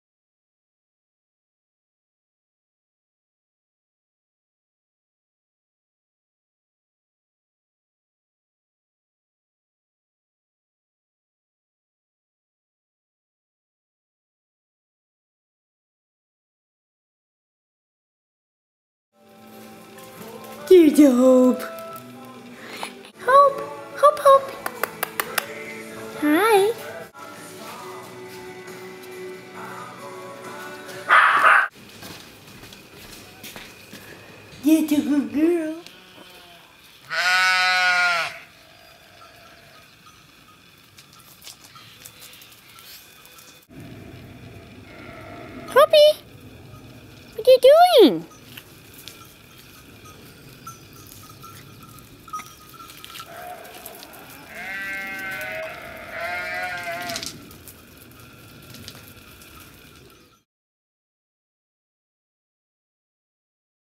Luxe's Hope
Hope is a spunky, sweet 3 lb, 8 oz, 1 year old, dark, babydoll faced Yorkshire Terrier. She is cuddly and calm, yet enjoys tagging along with you on outings. She is for sold. To learn more about Luxe Yorkies see our